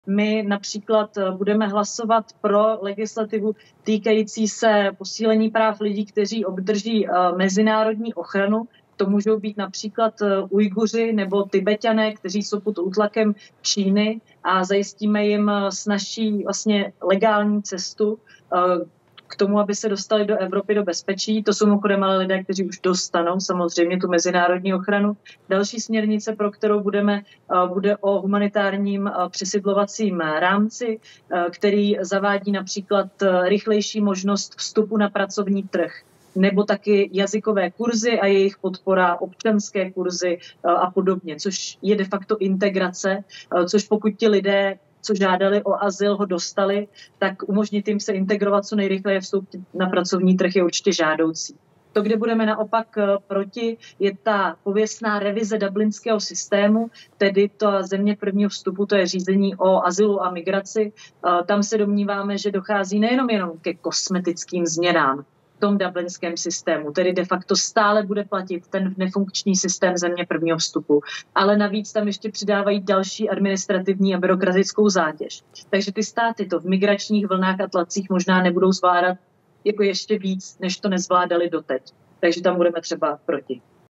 Migrační pakt: hlasování Pirátů
Migrační a azylový pakt. Horké téma posledních dní. Kde jsme hlasovali pro, kde proti, a proč? Výčet není vyčerpávající, ale věřím, že nastiňuje náš přístup: racionální a humanistický. Na ani jednom z nich neslevujeme.